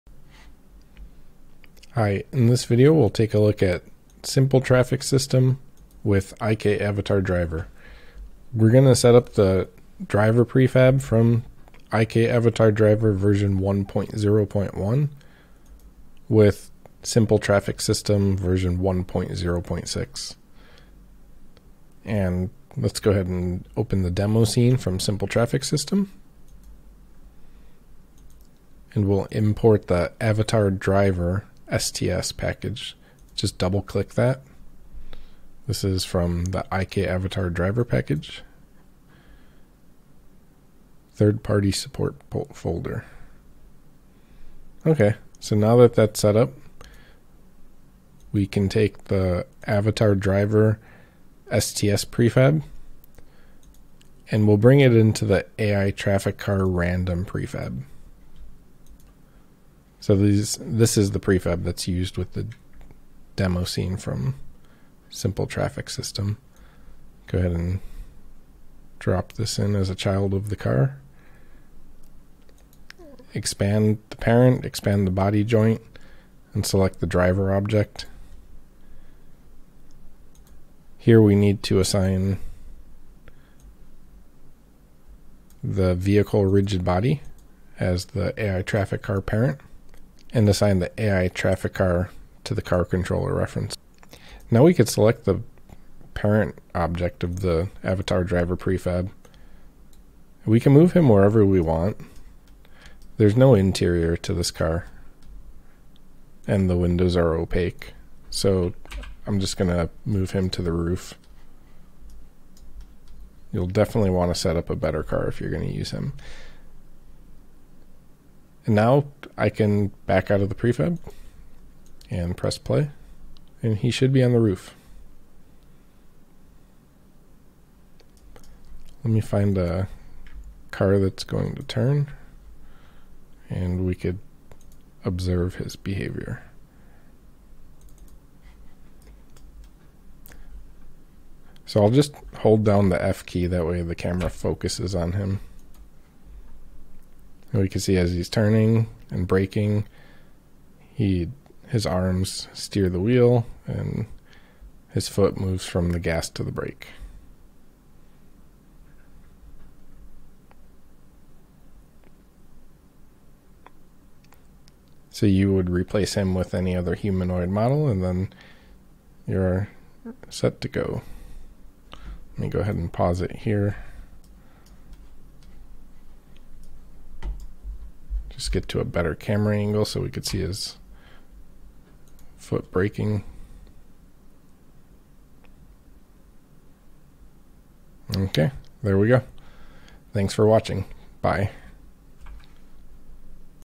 Unity IK Avatar Driver with STS - Simple Traffic System Tutorial
Don't let your cars drive empty!

IK Avatar Driver is an arcade style car controller with a procedurally animated inverse kinematics avatar driver, you can customize it with your own car and humanoid-character models. The avatar is modular and can be used with Simple Traffic System, Edy’s Vehicle Physics, NWH Vehicle Physics and Realistic Car Controller.

Key Car Controller Features:
• Mobile ready prefab (drag & drop)
• Look-back camera
• Driver-helmet camera
• Automatic or manual transmission
• Left & right side driver prefabs
• Rear & side view mirrors
• Nitro
• Quick prefab configuration for new car models

Key Avatar Features:
• Use any Humanoid Avatar model
• Driver works on left or right side of car
• Procedural animations include: steer, look in steer direction, shift, apply foot brake, gas and shift pedal
• Works with Simple Traffic System
• Works with Edy’s Vehicle Physics
• Works with NWH Vehicle Physics
• Works with Realistic Car Controller